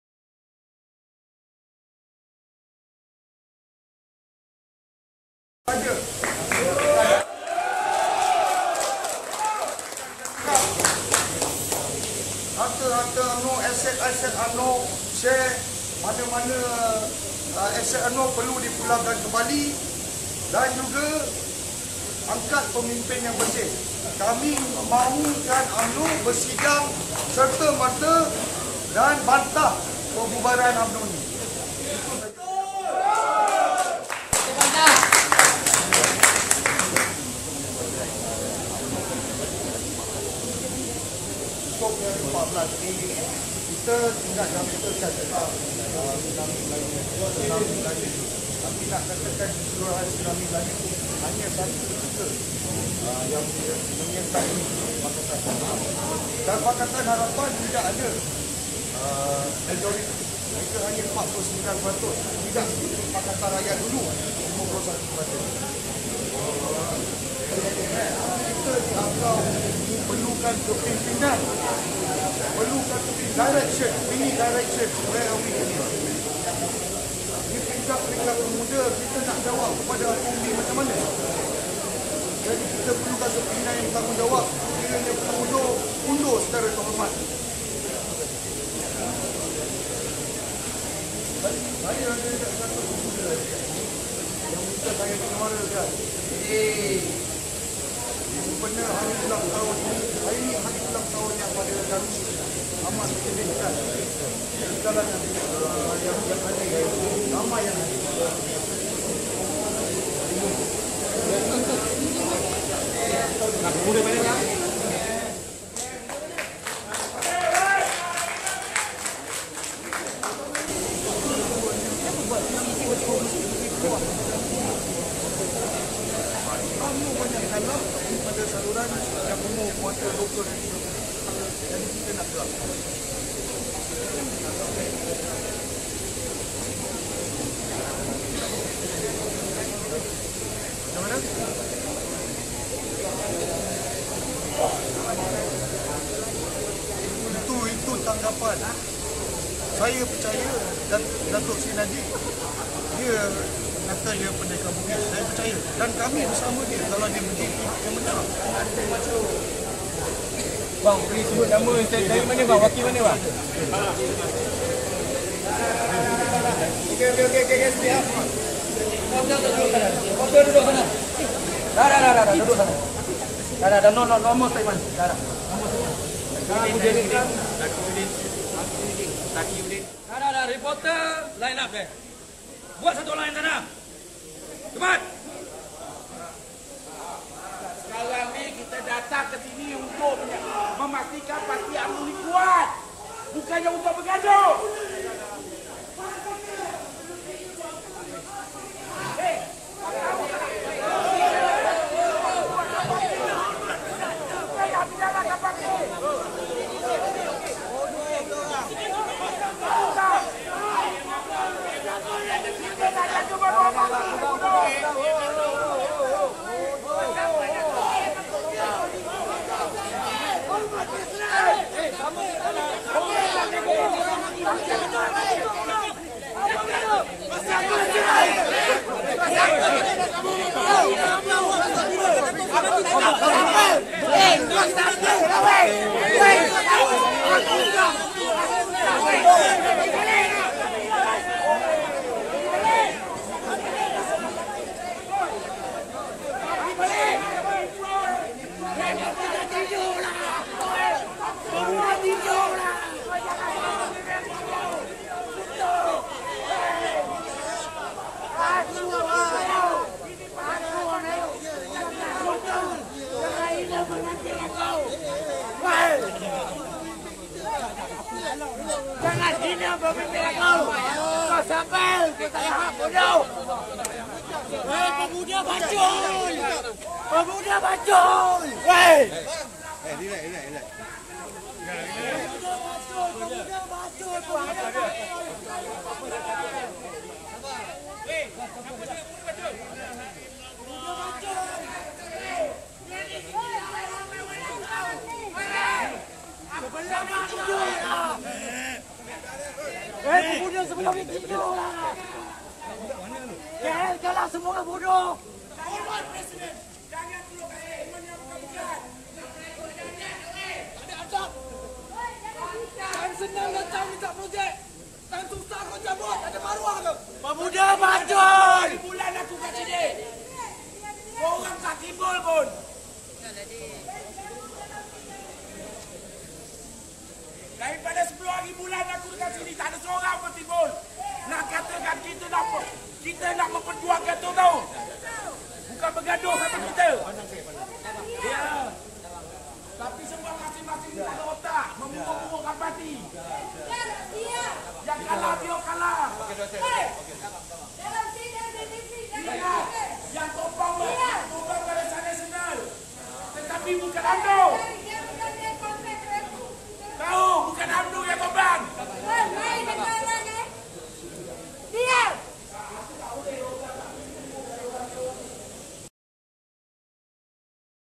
Kecoh Di Luar PWTC - Sambutan Ulangtahun UMNO KE 72
Kuala Lumpur 11/05/2018